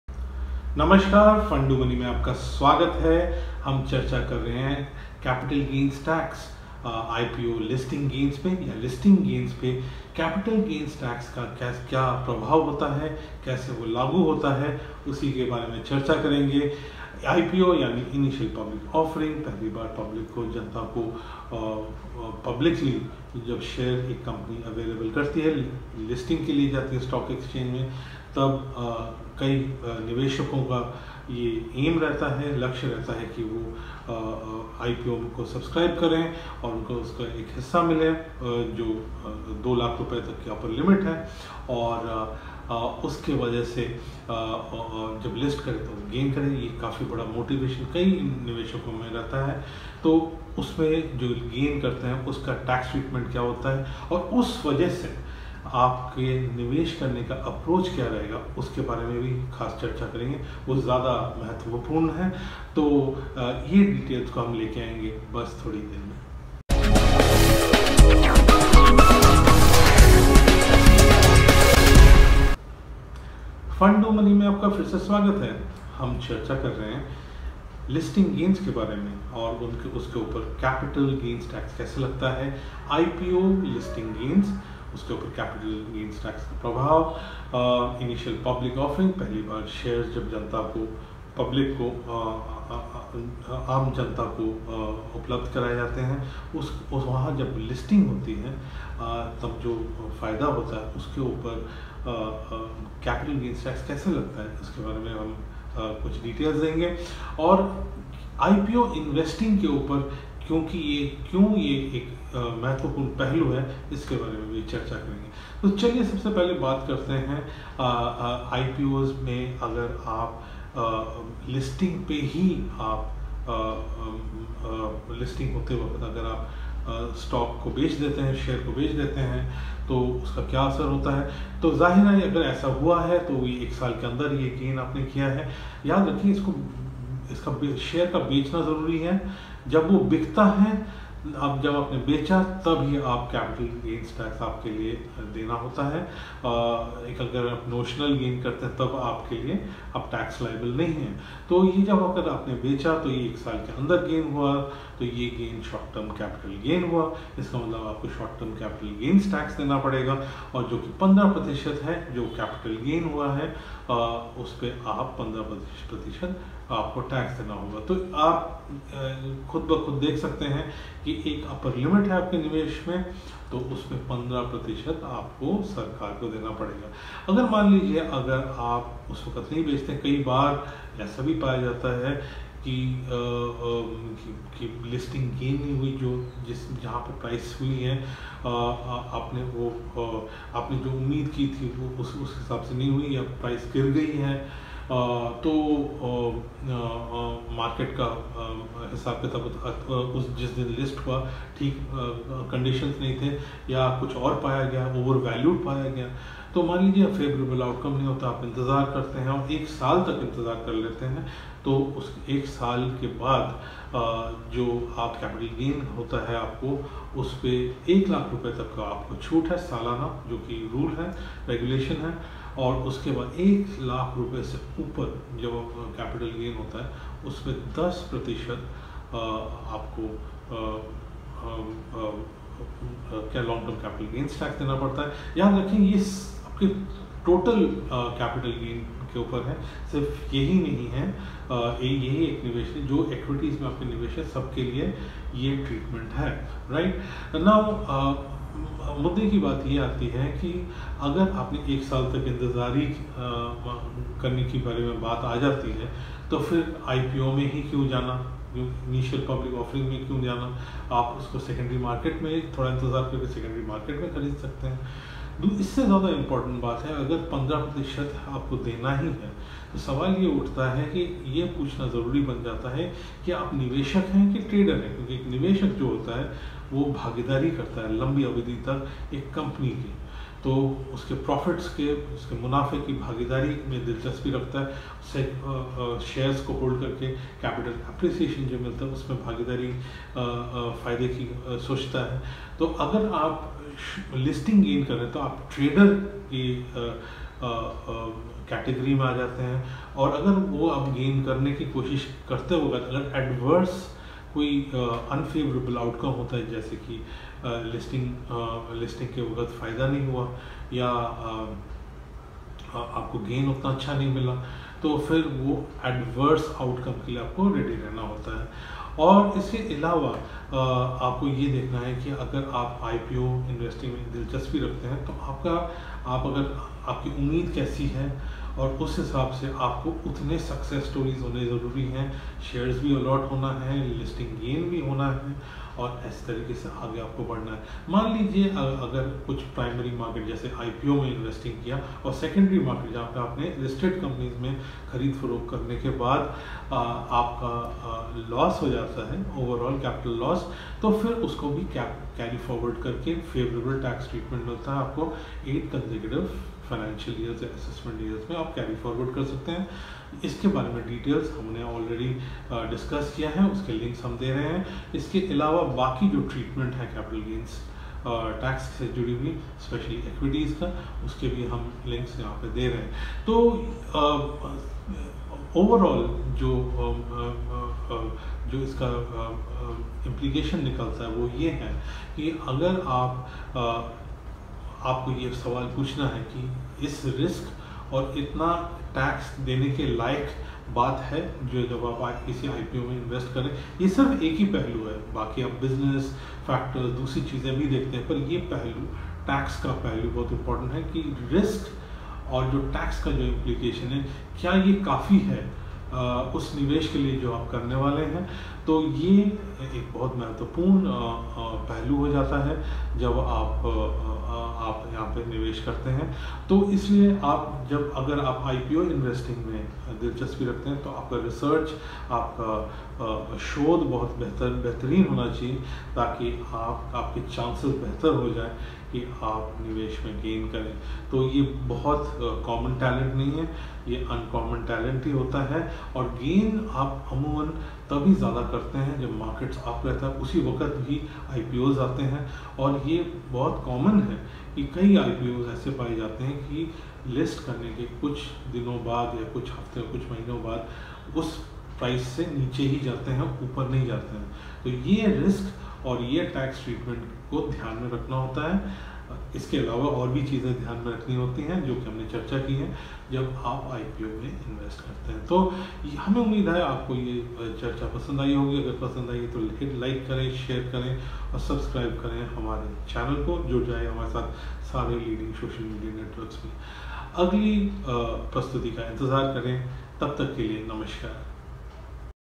Listing Gains पर Capital Gains Tax: Capital Gains Tax On Listing Gains
हम चर्चा करंगे listing gains पर capital gains tax के विषय में अथवा Capital Gains Tax On Listing Gains.
Video on "Tax on Gold Investments l सोने के निवेश पर आय कर  कैसे लगता है"
Edited Highlights
00:00 Chapter 1 Introduction
00:08 हम चर्चा करंगे listing gains पर capital gains tax के विषय में
00:14 IPO listing gains पर capital gains tax कैसे लागू होता है
00:36 Listing gains कई IPO निवेशकों का लक्ष्य होता है
00:41 किसी IPO में individual investor के निवेश की उपरी सीमा Rs 2 लाख है
00:58 Listing gains पर capital gains tax के कारण आपके निवेश की रणनीति क्या रहेगी इस विषय पर भी चर्चा करेंगे

1:13 Chapter 2 Listing Gains पर Capital Gains Tax
2:26 Listing पर share जब बेचा जाता है तब ही capital gains tax देने का कारण बनता है
2:36 क्योंकि यह shares 1 साल के अंदर बेचा गया है इसलिए यह short term capital gains है
2:39 इसका मतलब आपको 15% short term capital gains tax देना पड़ेगा
3:41 1 वर्ष से अधिक समय तक शेयर रखने के बाद बेचा गया तो capital gain को long term capital gain माना जायेगा
3:46 इस पर long term capital gains tax लगता है
3:55 सालाना Rs 1 lakh तक का long term capital gains पर tax नहीं लगता
3:57 इससे अधिक long term capital gains होने पर 10% long term capital gains tax देना पड़ता है
4:24 1 साल इंतेज़ार करने से बेहतर IPO निवेश न कर list होने के बाद share को खरीदना है
4:35 15% short term capital gains tax के नियम के कारण आपको निर्णय करने की आवश्यकता है कि आप निवेशक हैं कि trader
4:40 एक निवेशक एक कंपनी के मुनाफ़े की लम्बी अवधि तक भागीदारी की तरफ देखता है
4:49 निवेशक का लक्ष्य long term capital gains होता है
4:59 शीघ्र capital gains पाना आपको trader के category में ले आता है
5:18 आशा के अनुरूप capital gains नहीं मिलना या capital gains ही नहीं मिलने की जोखिम के लिए आपको तैयार रहना होता है
5:33 कामयाब IPO investing के लिए shares allot होना और listing gains पाना आवश्यक है
5:55 IPO और अन्य shares और equity funds निवेश में loss होने पर भी भविष्य के वर्षों में carry forward करके capital gains tax benefit पाया जा सकता है
6:34 IPO निवेश से पहले यह निर्णय लेना आवश्यक है कि क्या इतना risk लेना और tax देना आवश्यक है